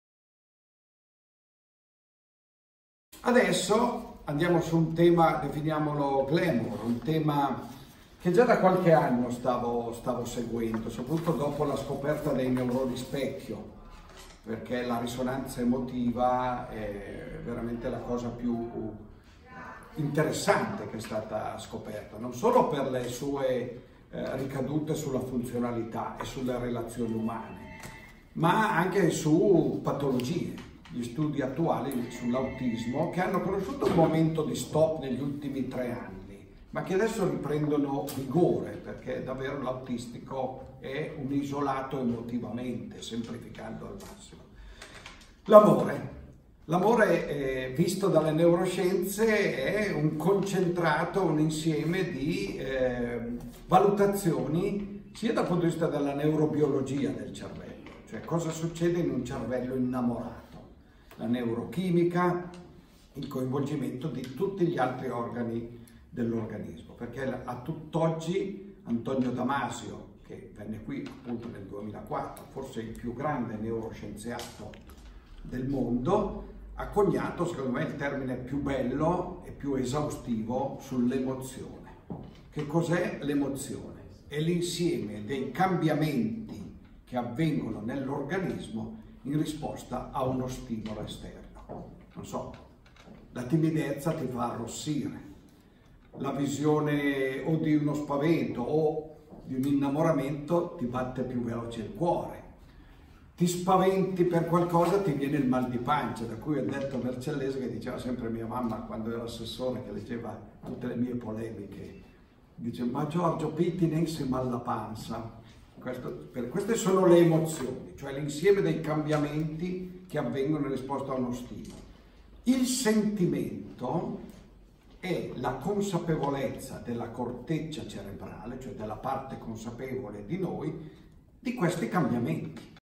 Amor che nella mente mi ragiona - Convegno Neuroscienze a Vercelli
Il dottor Pier Giorgio Fossale e la professoressa Paoletta Picco presentano presso la sede Ordine dei Medici di Vercelli il convegno di neuroscienze  Amor che nella mente mi ragiona Natura e cultura dell’amore in programma presso Aula magna “San Giuseppe”, Università degli studi del Piemonte Orientale a Vercelli, il 19 ottobre 2019.